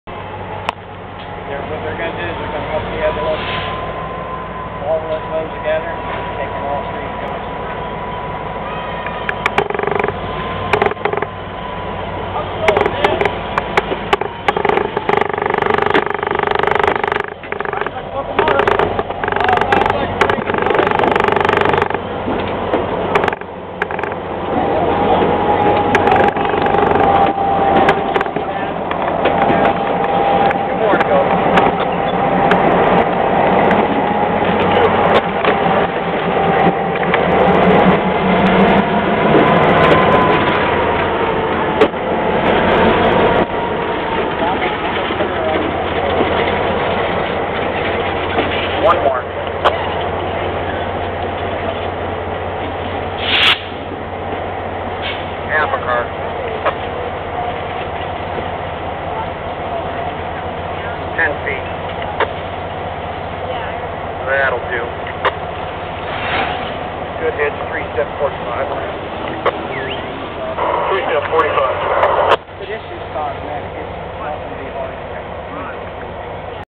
39 Sandwich
Knox and Kane #39 is sandwiched between GNWR 45 and BPRR 886 for the trip back to Johnsonburg.  #39 was purchased by the Georges Creek Railway to be restored by Gerald Altizer and his team back into her original Western Maryland Circus scheme.